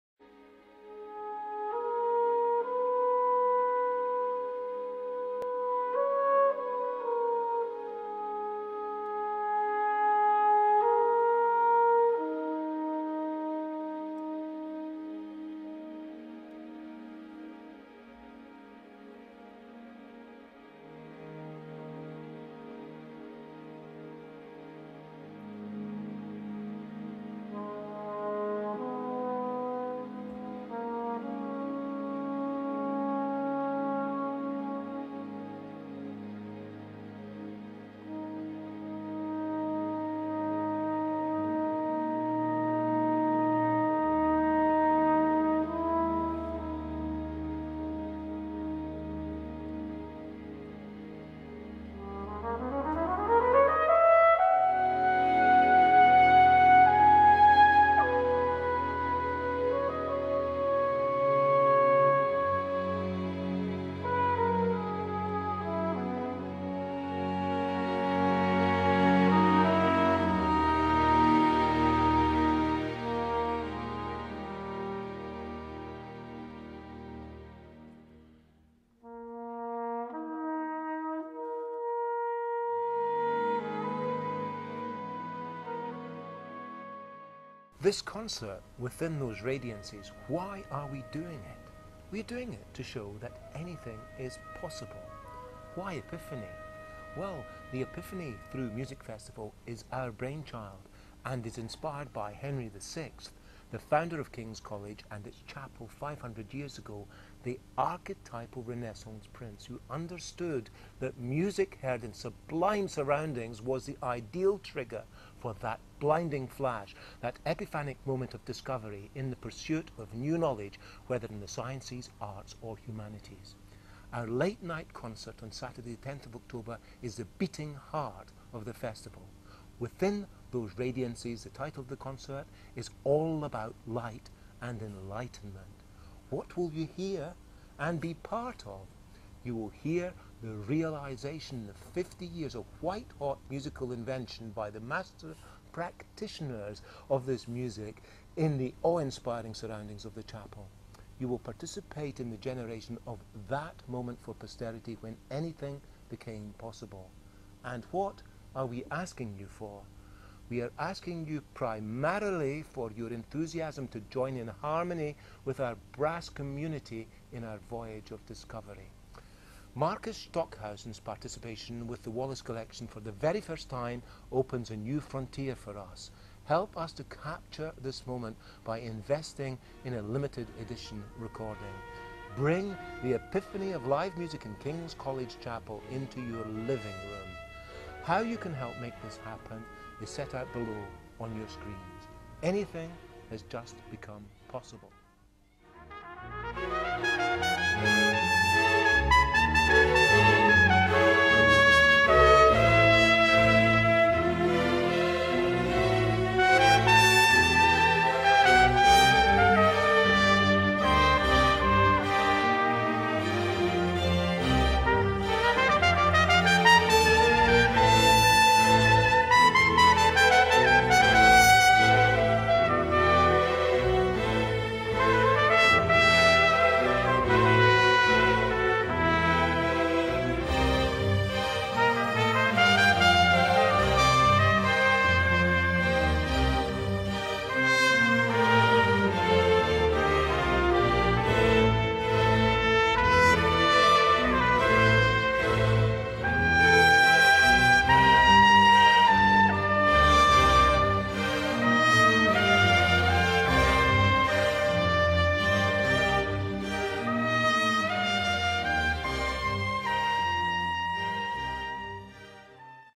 Crowd Funding - Brass and Electronica Fusion - A Legacy Recording Project
"Within Those Radiances ..." - a unique musical experience that needs to be captured and preserved for future generations.  Our crowd funding project can be seen - be part of something unique.

If you've watched the video, you'll already realise that you could be part of a significant musical legacy.  Helping us to embrace and capture for posterity, through audio and video recordings, a unique collection of performers and their performances amidst an histroic setting.

At 9.30pm on 10th October 2015 at King's College Chapel, Cambridge, the concert "Within Those Radiances ..." will commence.  It is the beating heart of the contemporary music festival Epiphany Through Music  (devised by John Wallace and Andrew Powell to be part of the King's 500 celebrations), and represents a magnificent union between contemporary music for brass and live electronics, its creative pioneers and today's advocate performers.

Help us to enable future musicians, scholars and enthusiasts to share and learn from this remarkable occasion - a matchless fusion of brass and live electronics presenting works of the master practitioners of the genre.

Your support will help to create and preserve a defining moment in the journey of musical discovery for the brass community, enabling it to be enjoyed and appreciated by future generations.

We want to capture and preserve this significant musical experience, a unique moment of contemporary music performance amidst an historic setting which has been the inspiration for ideas and creativity for five centuries.  If you can help, you have the opportunity of receiving an exclusive collector's copy of these recordings for your personal use.

We need to raise at least £1,500 to provide the necessary equipment, facilities and services for this to be achieved.